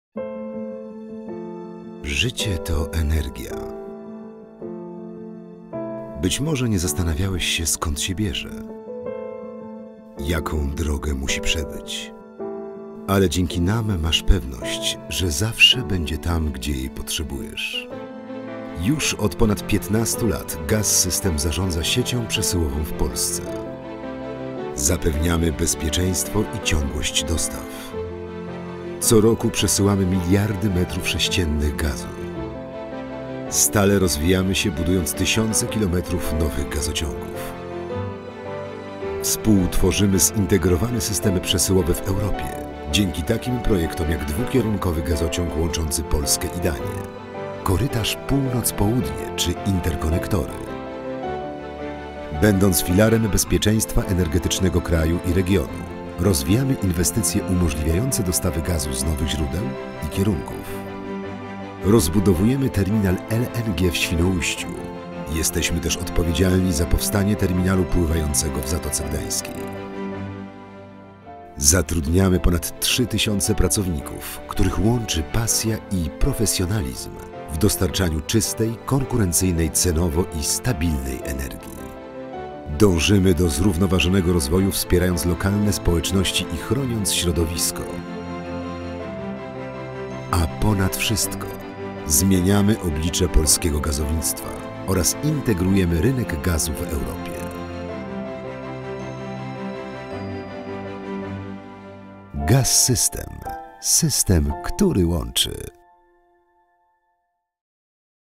GAZ-SYSTEM - system, który łączy
Firm korporacyjny GAZ-SYSTEM S.A. firmy strategicznej dla polskiej gospodarki oraz bezpieczeństwa energetycznego kraju.